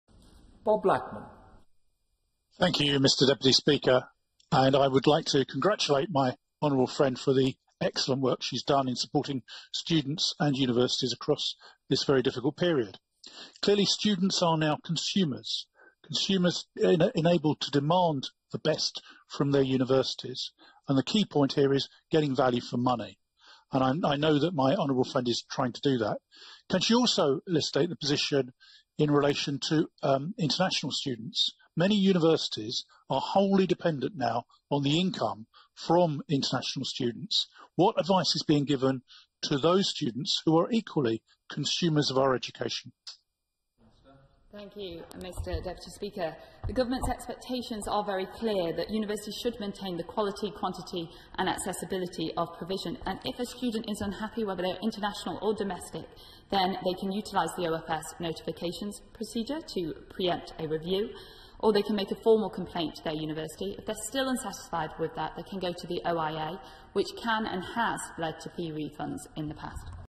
Bob Blackman asks about ensuring international students get value for money | 15 April 2021
Bob asks the Universities Minister about what advice can be given to international university students in the UK to ensure that they get value for money for their courses.

Bob asked this questions on 15 April 2021.